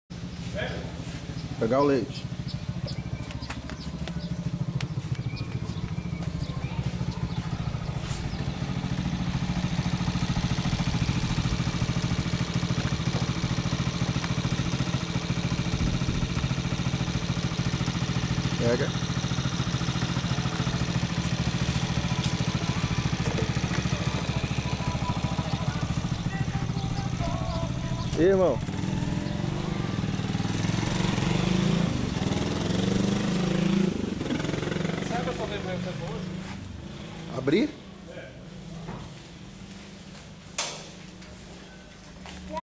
Ariadne recebendo o leitero
Manhã de domingo, Ariadne Gabriellen recebendo o leite na porta de casa.